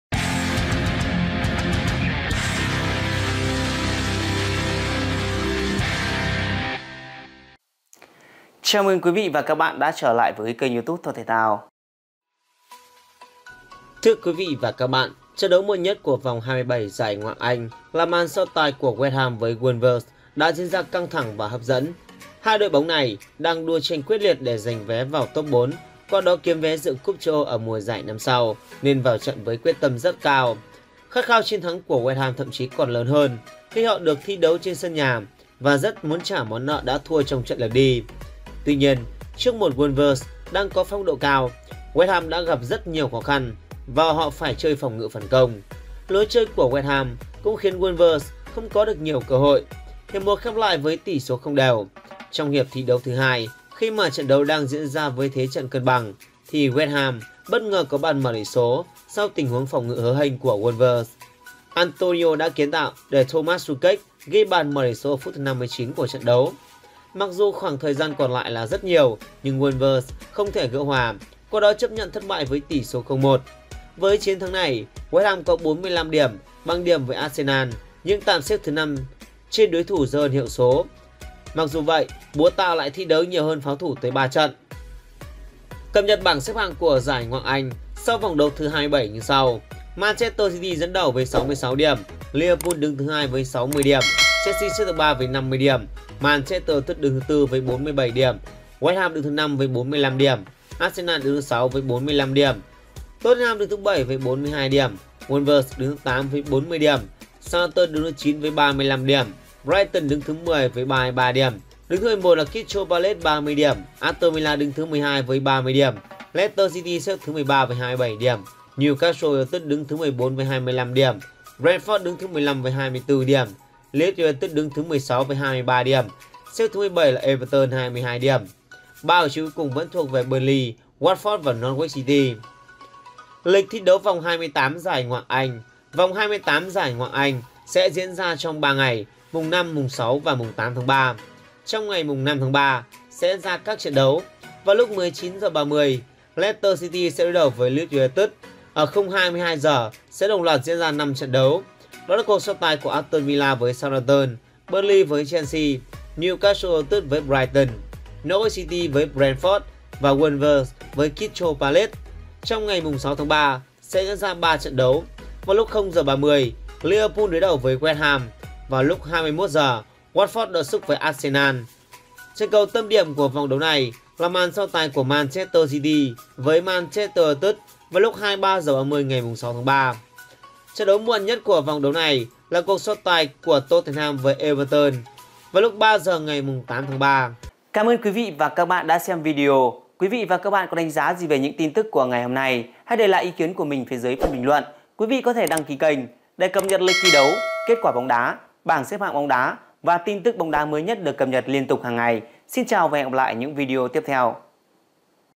Bảng xếp hạng Ngoại hạng Anh mới nhất | West Ham VƯỢT Arsenal, MU NGUY TO | Lịch thi đấu vòng 28
Bảng xếp hạng Ngoại hạng Anh mới nhất |  West Ham VƯỢT Arsenal, MU NGUY TO | Lịch thi đấu vòng 28 Premier League 2021/2022.